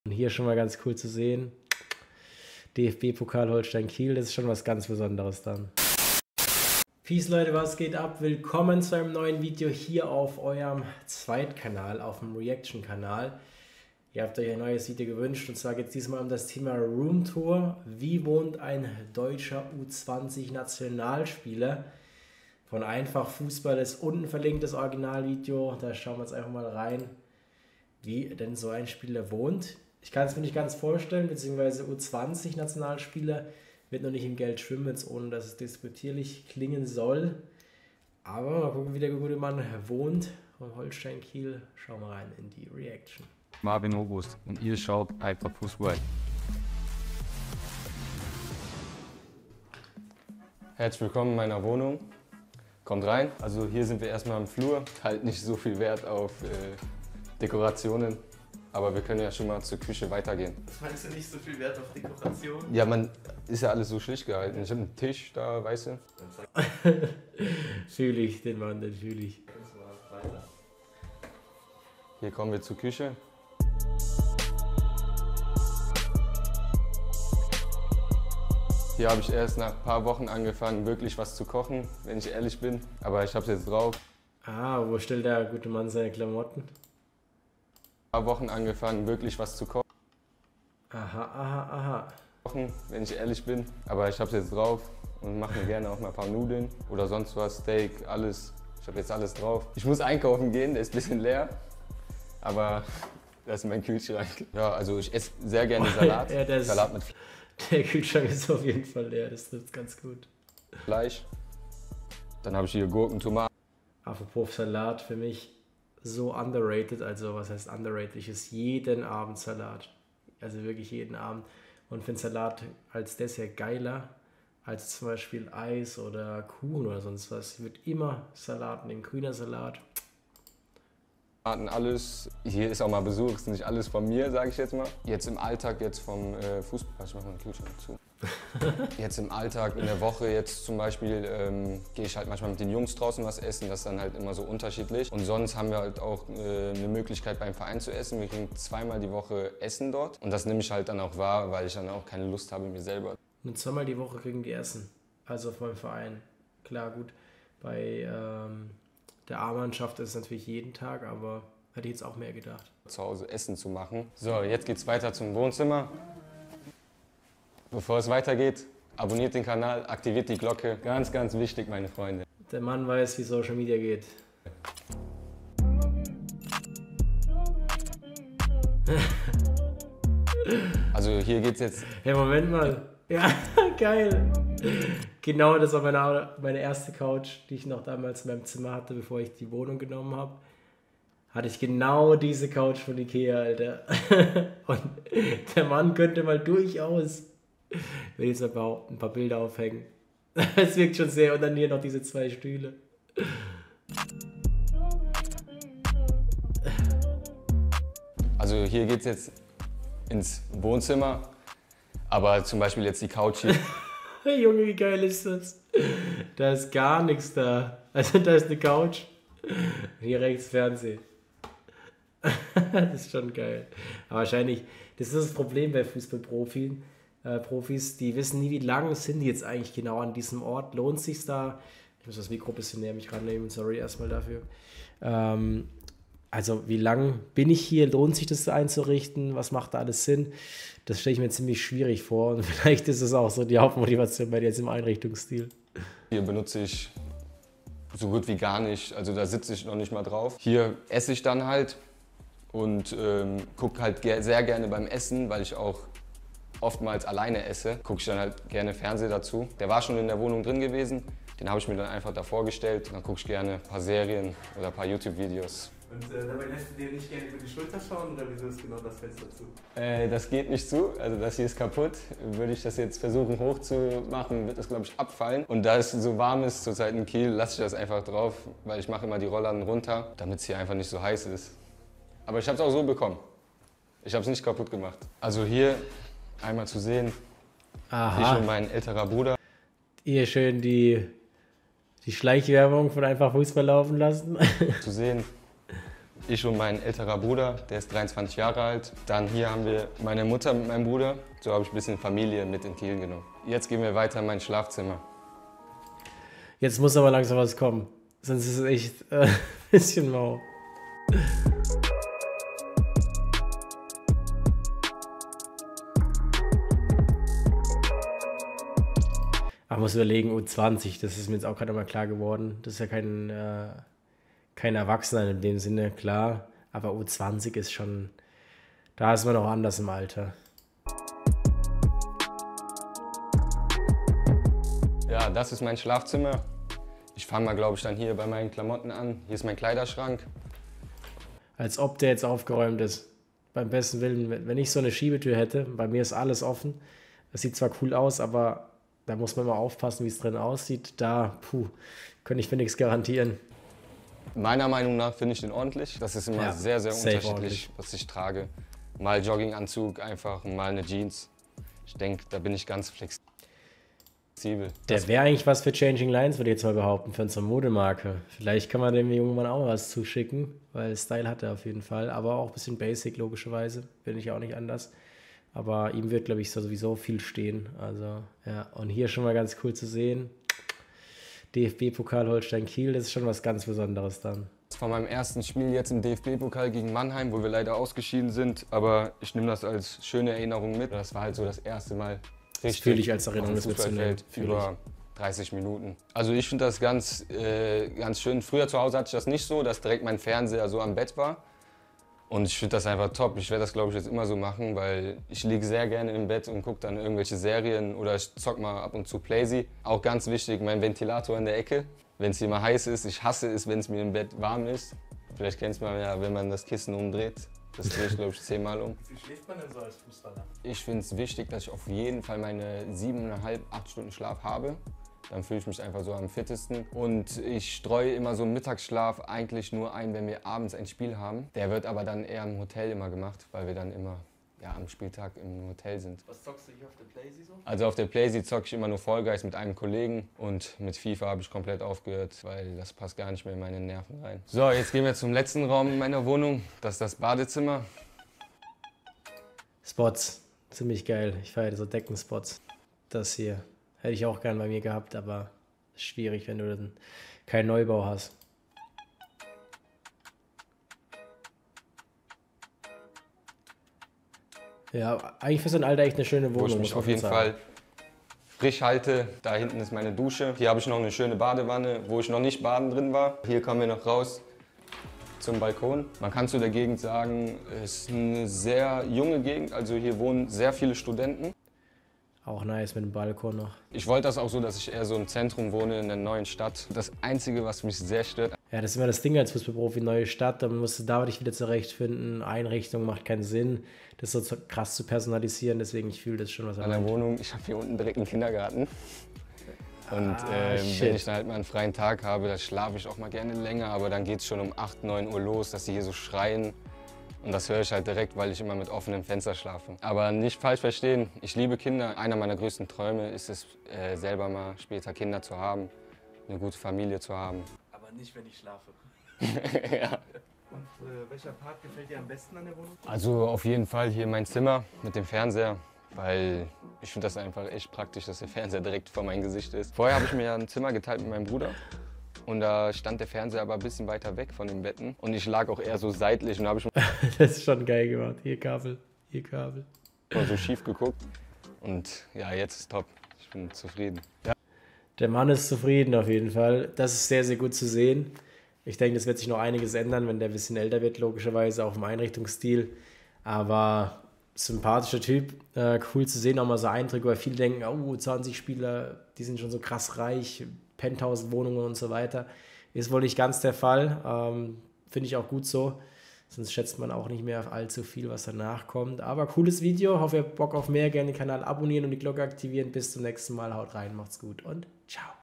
Wie wohnt ein Deutscher U20 Nationalspieler? - David Rau Reaction
Wie wohnt ein Deutscher U20 Nationalspieler? - David Rau Reaction! Willkommen zu einer neuen Reaction auf unserem Zweitkanal! Heute schauen wir uns die Roomtour eines Deutschen U20 Nationalspieler an.

Code: davidrau